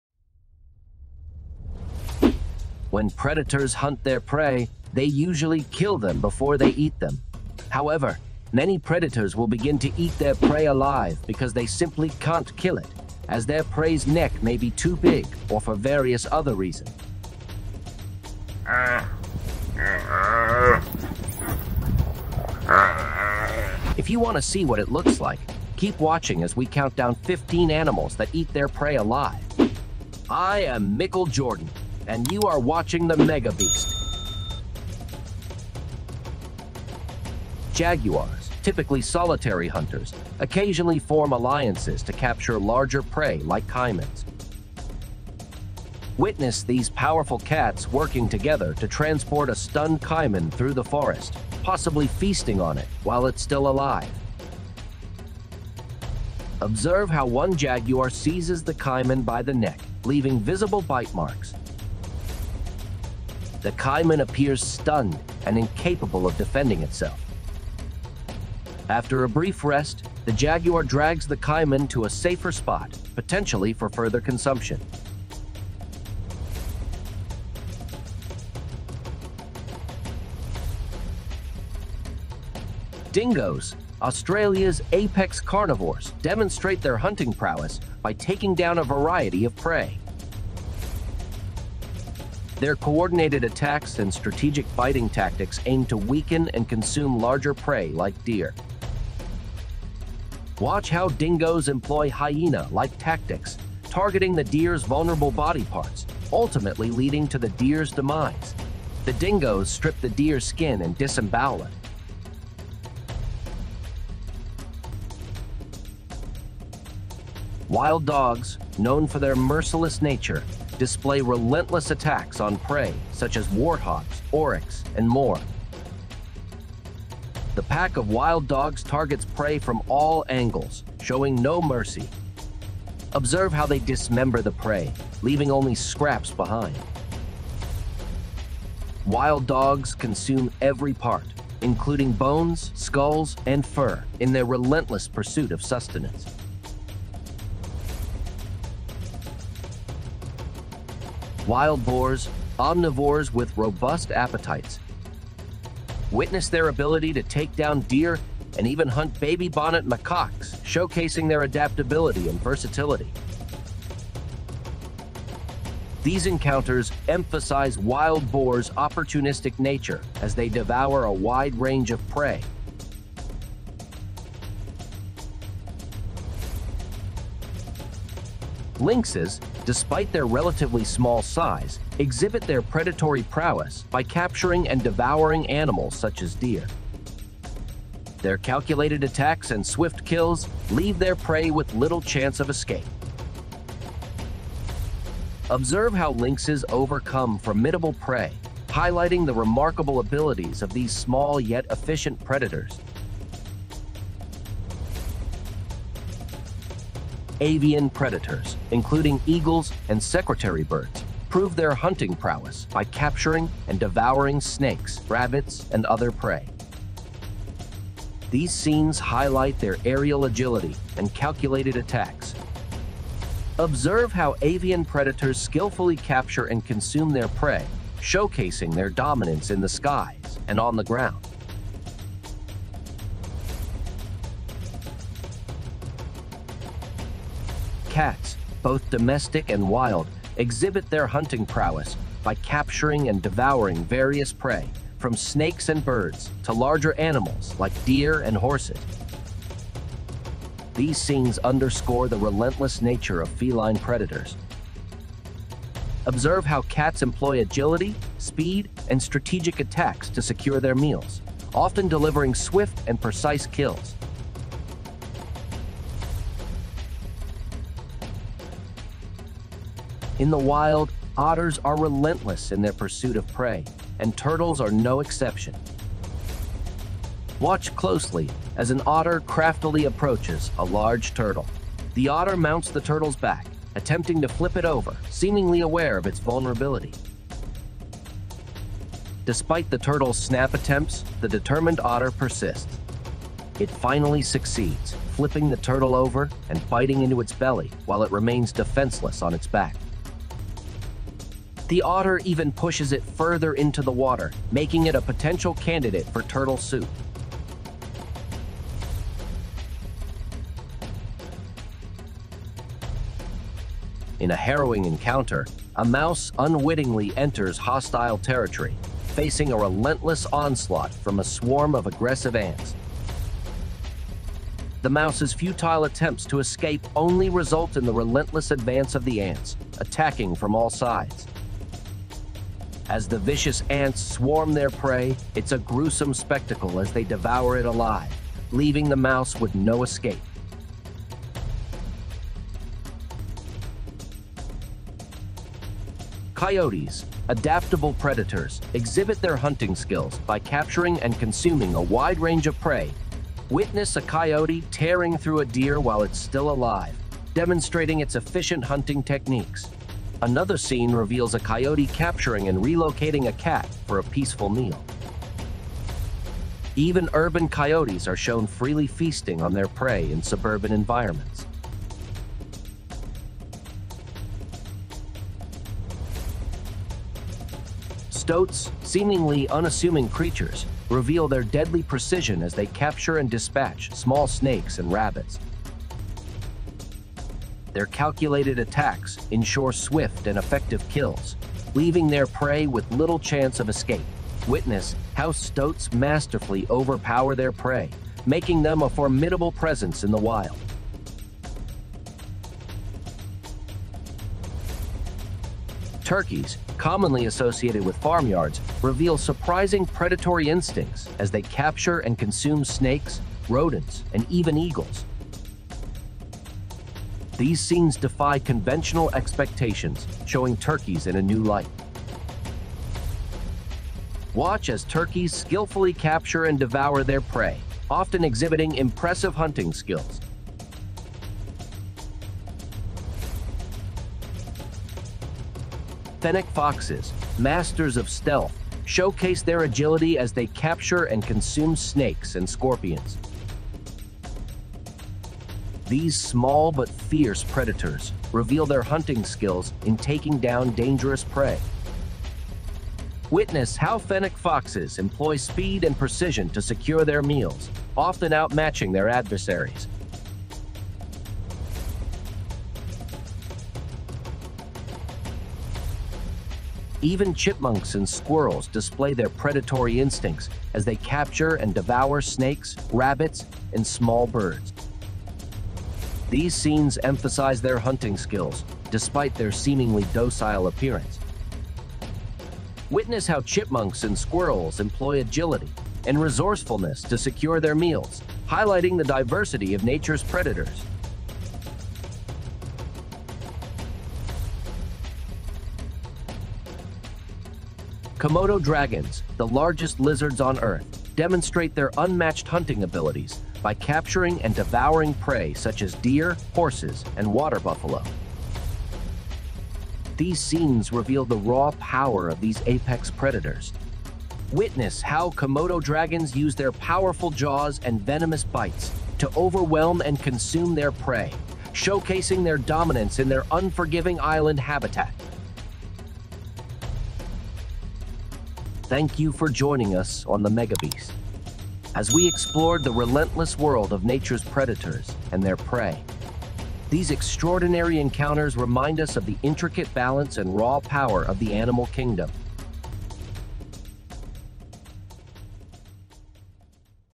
15 Savage Predators That Devour Their Prey ALIVE | Nature's Most Brutal Showdowns!
DESCRIPTION
In the animal kingdom, protecting their young is a top priority for many species. Therefore, it is common to see animals fighting with all their strength to defend their offspring from any threat. However, some animals do not have this same concern and, when attacking offspring of other species, end up getting hurt.

. WELLCOME.

Number 15
In the wild, otters are relentless in their pursuit of prey, and turtles are no exception. Watch closely as an otter craftily approaches a large turtle. The otter mounts the turtle's back, attempting to flip it over, seemingly aware of its vulnerability.

Number 14
Jaguars, typically solitary hunters, occasionally form alliances to capture larger prey like caimans. Witness these powerful cats working together to transport a stunned caiman through the forest, possibly feasting on it while it's still alive.

Number 13
In a harrowing encounter, a mouse unwittingly enters hostile territory, facing a relentless onslaught from a swarm of aggressive ants. The mouse's futile attempts to escape only result in the relentless advance of the ants, attacking from all sides.

Number 12
Dingoes, Australia's apex carnivores, demonstrate their hunting prowess by taking down a variety of prey. Their coordinated attacks and strategic biting tactics aim to weaken and consume larger prey like deer.

Number 11
Coyotes, adaptable predators, exhibit their hunting skills by capturing and consuming a wide range of prey. Witness a coyote tearing through a deer while it's still alive, demonstrating its efficient hunting techniques.

Number 10
Wild dogs, known for their merciless nature, display relentless attacks on prey such as warthogs, oryx, and more. The pack of wild dogs targets prey from all angles, showing no mercy.

Number 9
Stoats, seemingly unassuming creatures, reveal their deadly precision as they capture and dispatch small snakes and rabbits. Their calculated attacks ensure swift and effective kills, leaving their prey with little chance of escape.

Number 8
Wild boars, omnivores with robust appetites, occasionally engage in remarkable hunts. Witness their ability to take down deer and even hunt baby bonnet macaques, showcasing their adaptability and versatility.

Number 7
Turkeys, commonly associated with farmyards, reveal surprising predatory instincts as they capture and consume snakes, rodents, and even eagles. These scenes defy conventional expectations, showing turkeys in a new light.

Number 6
Lynxes, despite their relatively small size, exhibit their predatory prowess by capturing and devouring animals such as deer. Their calculated attacks and swift kills leave their prey with little chance of escape.

Number 5
Fennec foxes, masters of stealth, showcase their agility as they capture and consume snakes and scorpions. These small but fierce predators reveal their hunting skills in taking down dangerous prey.

Number 4
Avian predators, including eagles and secretary birds, prove their hunting prowess by capturing and devouring snakes, rabbits, and other prey. These scenes highlight their aerial agility and calculated attacks.

Number 3
Even chipmunks and squirrels display their predatory instincts as they capture and devour snakes, rabbits, and small birds. These scenes emphasize their hunting skills despite their seemingly docile appearance.

Number 2
Cats, both domestic and wild, exhibit their hunting prowess by capturing and devouring various prey, from snakes and birds to larger animals like deer and horses. These scenes underscore the relentless nature of feline predators.

Number 1
Komodo dragons, the largest lizards on Earth, demonstrate their unmatched hunting abilities by capturing and devouring prey such as deer, horses, and water buffalo. These scenes reveal the raw power of these apex predators.